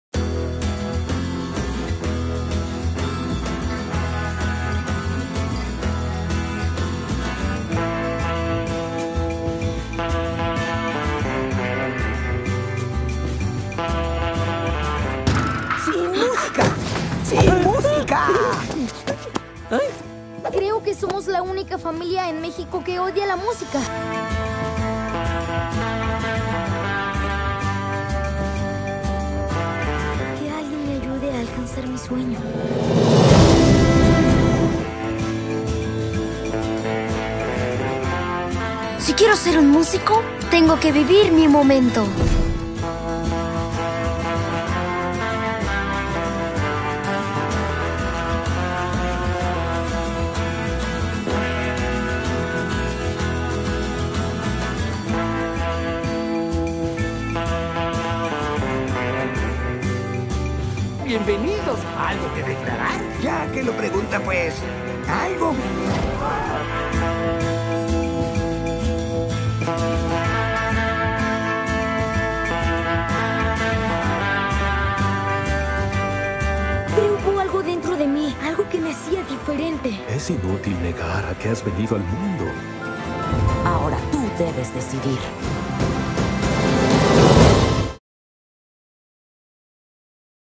Revive la película de 'Coco' | VERNE MÉXICO
La ruta del Día de Muertos por los escenarios de México que inspiraron la película ‘Coco’.